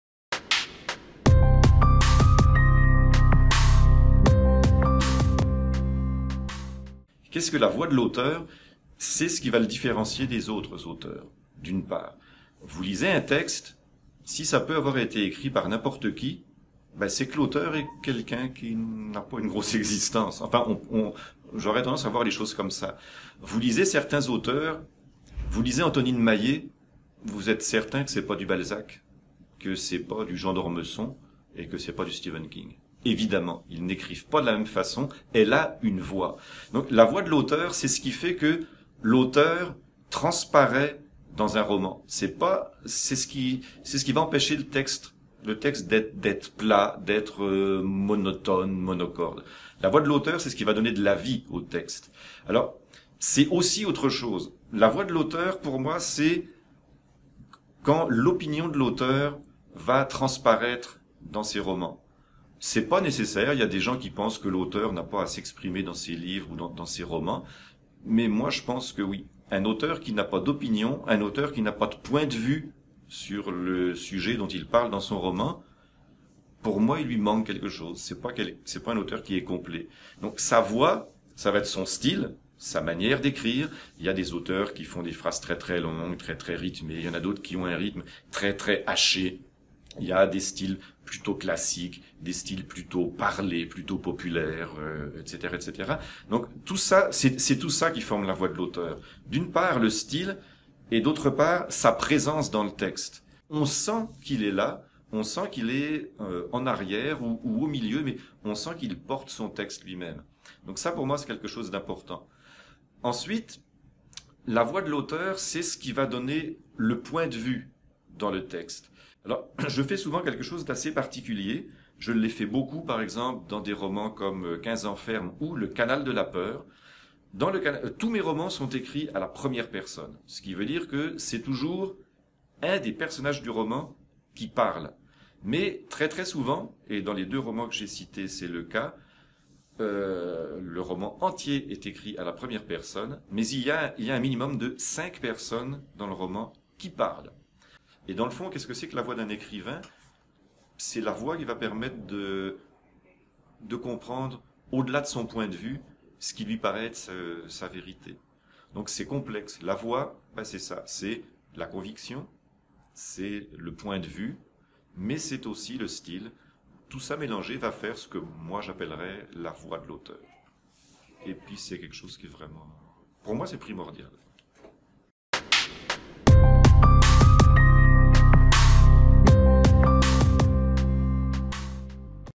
Qu'est-ce que la voix de l'auteur?
Entretien avec M. Laurent Chabin, romancier.